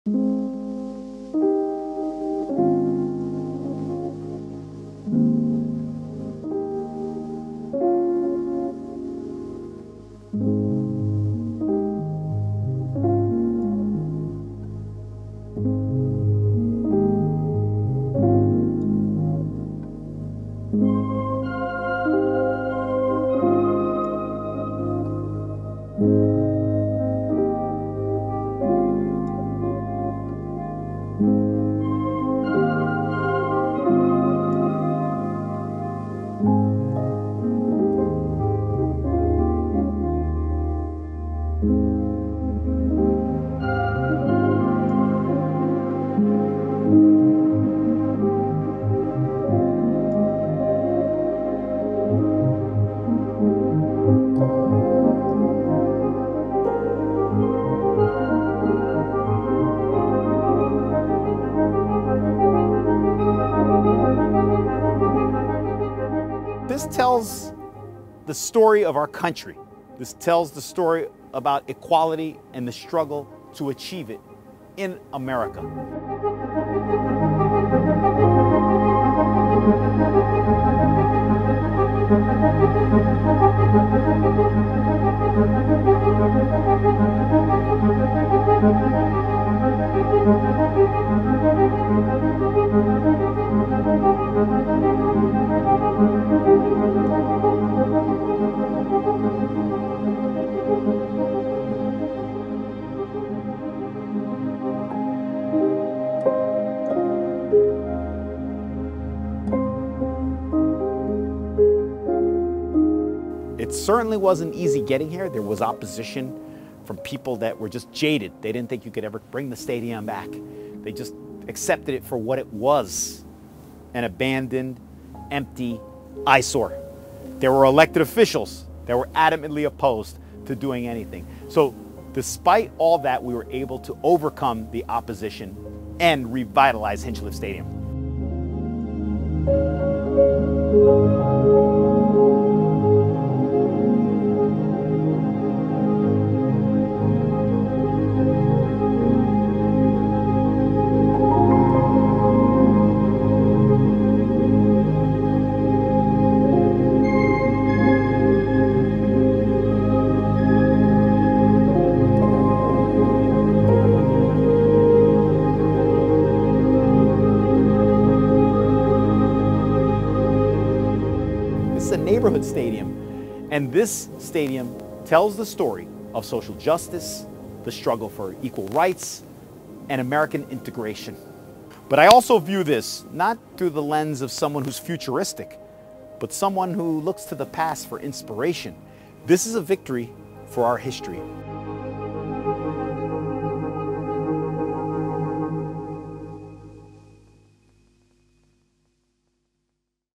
N.J.’s Negro League stadium transformed following renovation
New Jersey is home to a historic stadium that once hosted Negro league baseball games. Hinchliffe Stadium was once condemned but a major renovation has protected this important piece of history.

(Produced by Andre Malok)